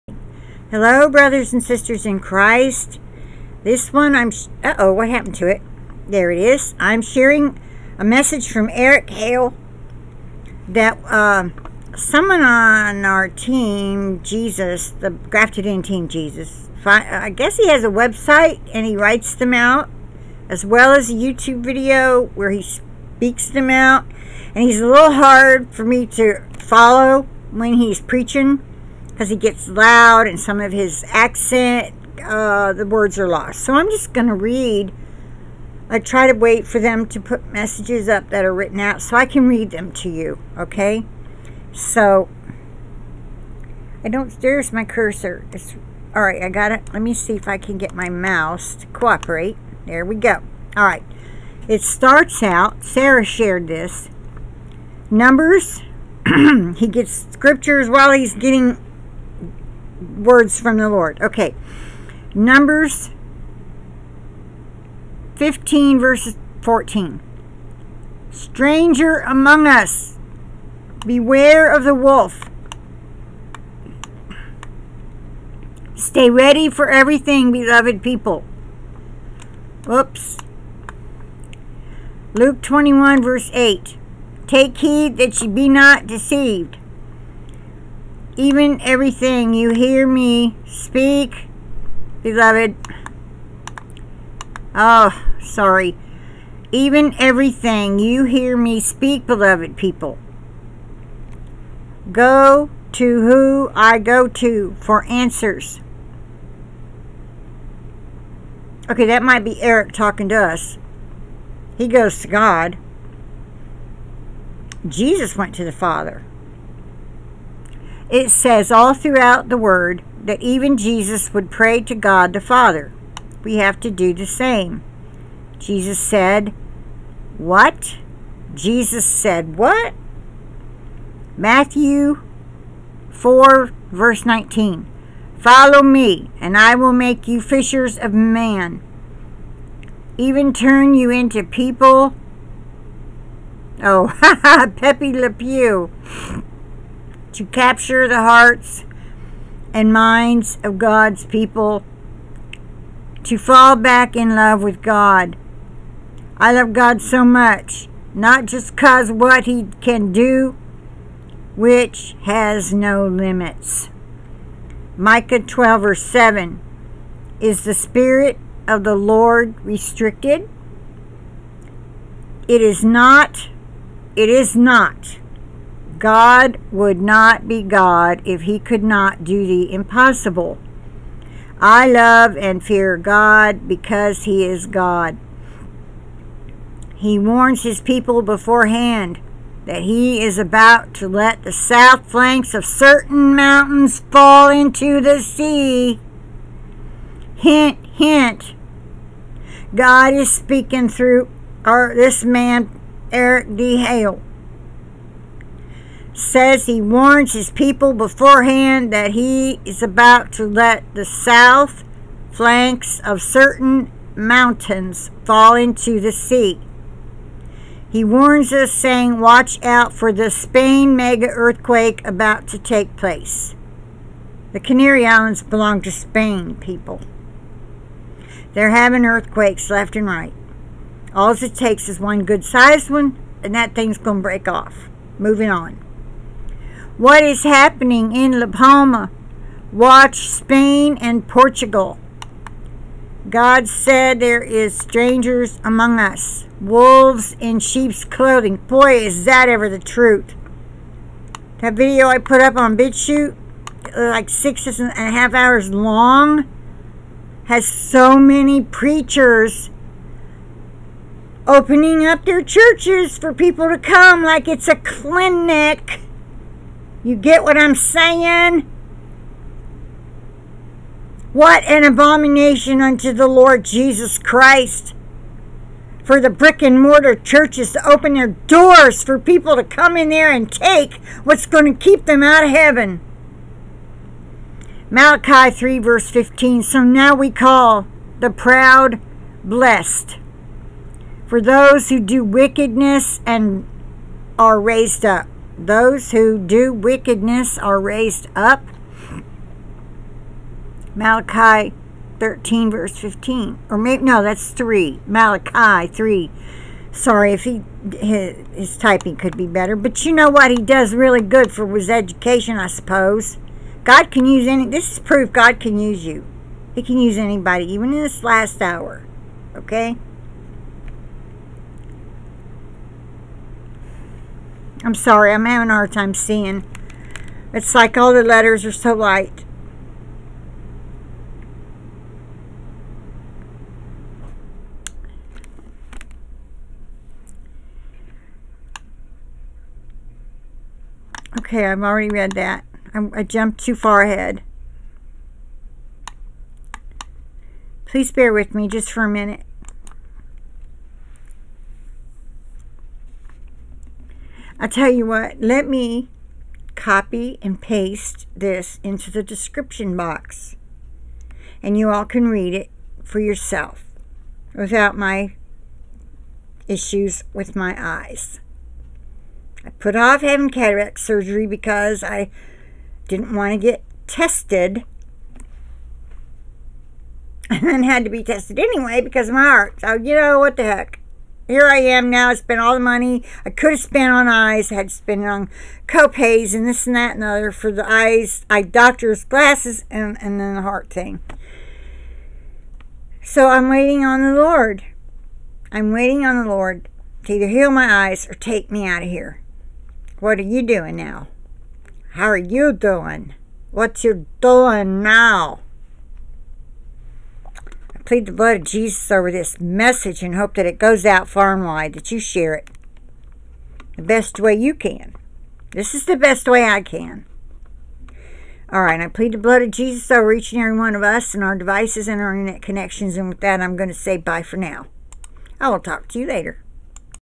NUMBERS 15v14 "STRANGER AMONG US!"

BEWARE OF THE WOLF,

STAY READY FOR EVERYTHING BELOVED PEOPLE,

LUKE 21v8 TAKE HEED THAT YOU BE NOT DECEIVED.

EVEN EVERYTHING YOU HEAR ME SPEAK BELOVED PEOPLE,

GO TO WHO I GO TO FOR ANSWERS,  IT SAYS ALL THROUGHOUT THE WORD, THAT EVEN JESUS WOULD PRAY TO GOD THE FATHER.

WE HAVE TO DO THE SAME, JESUS SAID WHAT?

MATTHEW 4v19  FOLLOW ME, AND I WILL MAKE YOU FISHERS OF MAN,

EVEN TURN YOU INTO PEPE LE PEW, TO CAPTURE THE HEARTS AND MINDS OF GOD'S PEOPLE TO FALL BACK IN LOVE WITH GOD,

I LOVE GOD SO MUCH, NOT JUST CAUSE WHAT HE CAN DO WHICH HAS NO LIMITS,

MICAH 2v7 IS THE SPIRIT OF THE LORD RESTRICTED?

IT IS NOT, GOD WOULD NOT BE GOD IF HE COULD NOT DO THE IMPOSSIBLE,

I LOVE AND FEAR GOD BECAUSE HE IS GOD,

HE WARNS HIS PEOPLE BEFORE HAND THAT HE ABOUT TO LET THE SOUTH FLANKS OF CERTAIN MOUNTAINS FALL INTO THE SEA,

HE WARNS US SAYING WATCH OUT FOR THE SPAIN MEGA EARTHQUAKE ABOUT TO TAKE PLACE,

WHAT IS HAPPENING IN LA P@LM@ WATCH SPAIN AND PORTUGAL,

GOD SAID THERE IS STRANGERS AMONG US, WOLVES IN SHEEP'S CLOTHING,

MALACHI3v15 SO NOW WE CALL THE PROUD BLESSED, FOR THOSE WHO DO WICKEDNESS ARE RAISED UP,

THEY SHALL BE BROUGHT DOWN WITH THE PRIDE AND TRICKERY OF THEIR OWN HANDS,

2nd CHRONICLES 19 "SHOULD YOU HELP THE WICKED AND LOVE THOSE WHO HATE THE LORD?

I WILL NOT IN JESUS NAME,

IT IS SETTLED IN MY HEART,

JOB 6v6  CAN FLAVORLESS FOOD BE EATEN WITHOUT SALT?  OR IS THERE ANY TASTE IN THE WHITE OF EGG,

v7 MY SOUL REFUSES TO TOUCH THEM:THEY ARE A LOATHSOME FOOD TO ME,

I'M PRAYING FOR ALL Y'ALL IN JESUS NAME AS WELL AS PRAYING FOR ACTUAL RAIN TO FALL ON MY STATE,

OUT THE SAME MOUTH I'M DECLARING THE WRATH OF GOD UPON THE EARTH,  JUDGMENT FOR SIN,

EVERYTHING THAT IS COMING THE DESTRUCTION OF LAS VEGAS I SAW AGAIN, THEY CALL IT SIN CITY BECAUSE THEY KNOW ITS FULL OF SIN IN TOTAL REBELLION AGAINST GOD,

YOUR GOING TO KNOW EXACTLY WHEN WE WILL DO A LOVE-STREAM 7.+ MAGNITUDE EARTHQUAKE THAT IS ABOUT TO STRIKE,

THAT IS THE CHOSEN SIGN FOR THE FINAL WARNINGS TO BEGIN TO BE DISTRIBUTED OUT GLOBALLY BEFORE GOD TEAR THIS THANG UP,

ITS SEVERAL MASS MIGRATIONS OF HUMAN BEINGS COMING, I T WAS PEOPLE ON THE NEWS IN A DREAM SAYING THEY WERE LEAVING BECAUSE THE MAGMA WAS DESTROYING PLACES AND THEY KEPT SAYING,  "THE TSUNAMIS ARE COMING, THE TSUNAMIS ARE COMING GET TO HIGHER GROUND!"

THIS WORLD IS ABOUT TO GET IT'S,

EZEKIEL23v20 "ISSUE!"

IT WILL BE HISTORIC PROPHECY FULFILLMENT, SWIFTLY AND SPEEDILY, AS IF PIERCING THROUGH THE WINDOW OF TIME.

1st CORINTHIANS 13v12 "DIMLY!"